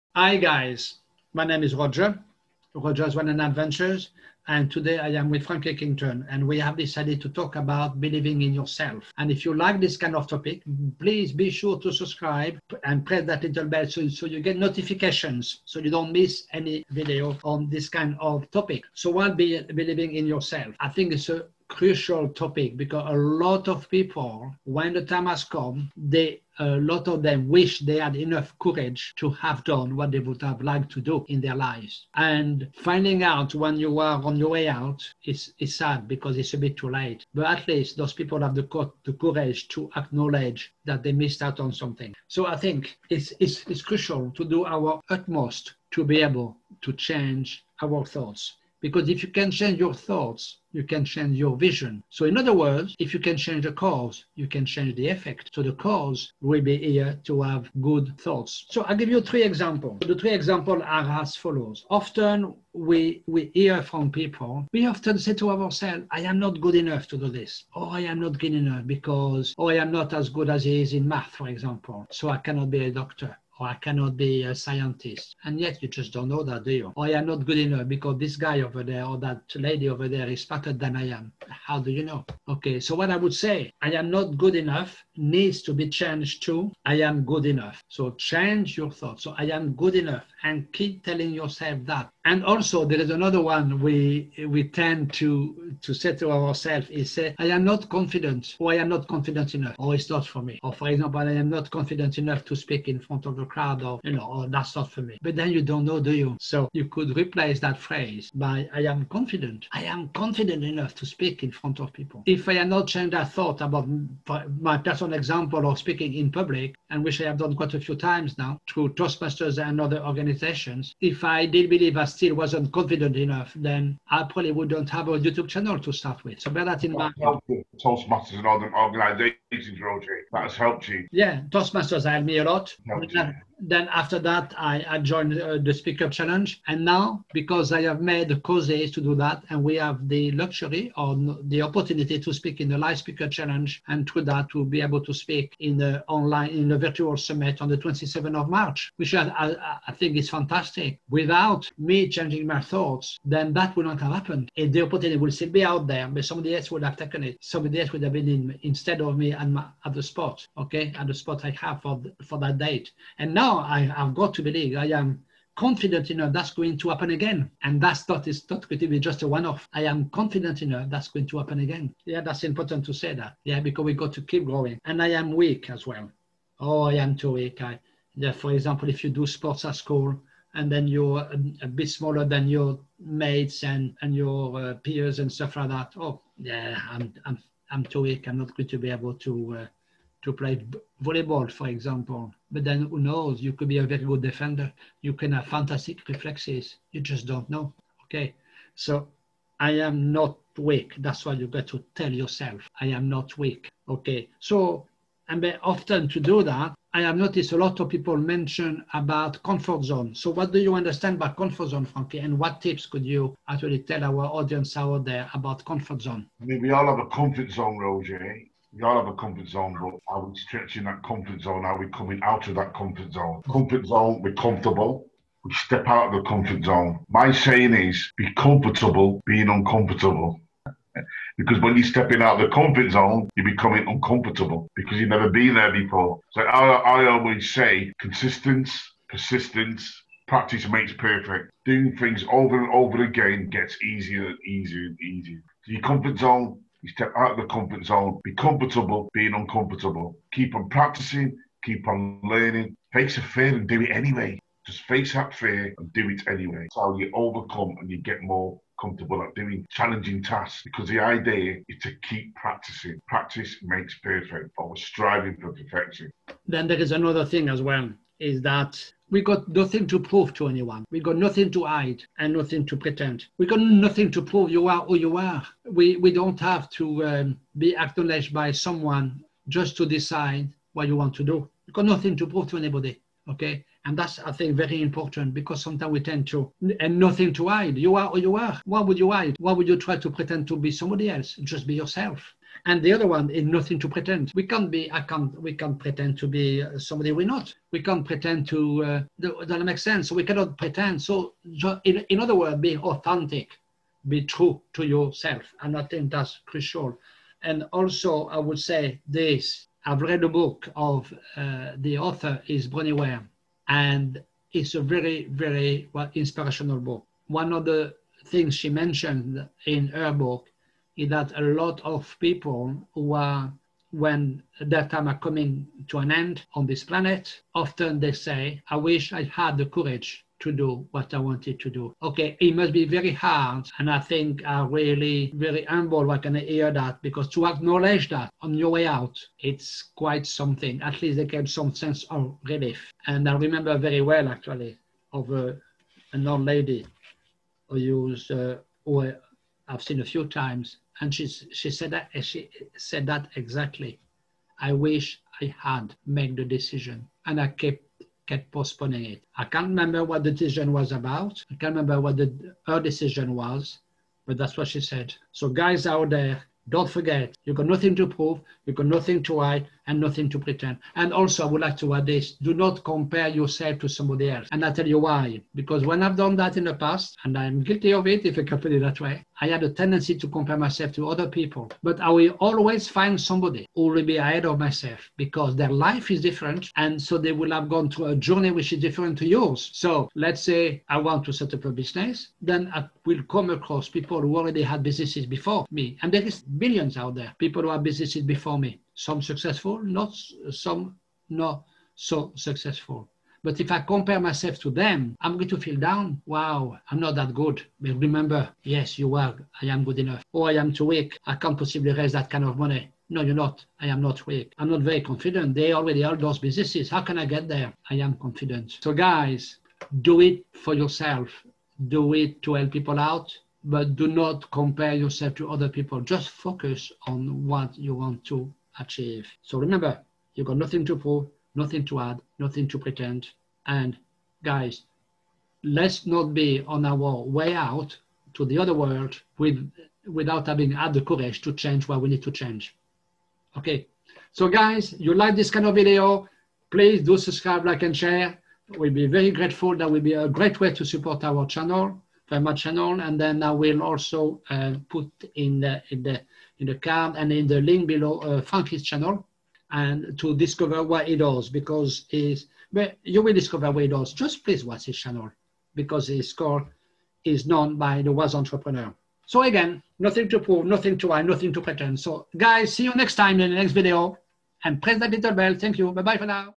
BELIEVING IN YOURSELF - 3 POWERFUL TIPS
One of the secrets to believing in yourself lies in the ability to change your thoughts. One of the ways to do so is to live outside your comfort zone. This is when the magic happens. You need to become comfortable at being uncomfortable.